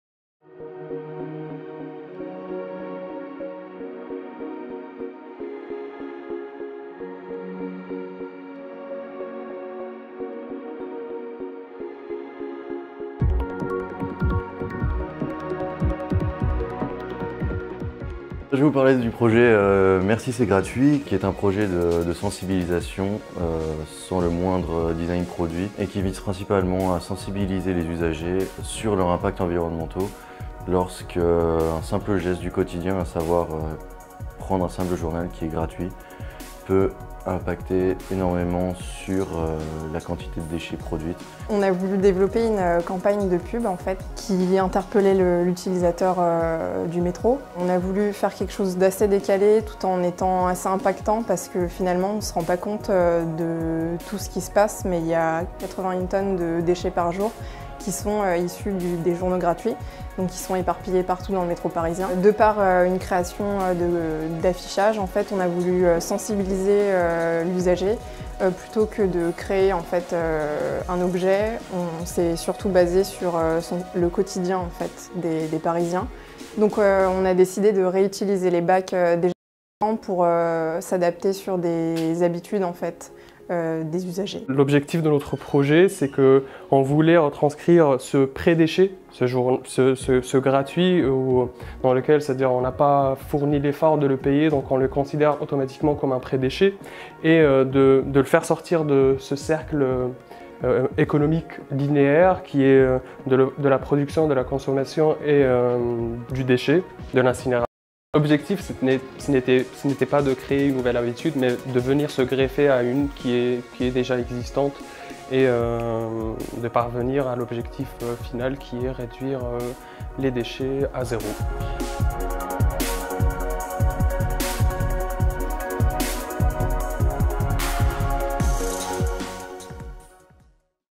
Concours Design Zero Dechet 2018 - 3e Prix - Bachelor Design Global Eco-responsable
Nos étudiants du Bachelor Design Global Éco Responsable primés à l’occasion du concours zéro déchet !

Pour cette 6e édition, le Syctom a proposé aux participants du concours Design Zéro Déchet de réfléchir à la prévention et la gestion des déchets en milieu urbain, espaces publics et lieux de transit. Les projets pouvaient aussi bien porter sur la réduction des déchets générés dans ces lieux que sur leur collecte et leur traitement  afin de les inscrire dans une boucle production/consommation/valorisation plus vertueuse.

Félicitations à Abdel Jalel Chib, Alexandre Geffroy et Maeva Guyon qui ont reçu le 3e prix avec leur projet intitulé « Merci c’est gratuit ». Les étudiants se sont vu remettre la somme de 1000 euros par M. Jean-Sébastien Blanc, Co-fondateur de 5.5 designstudio.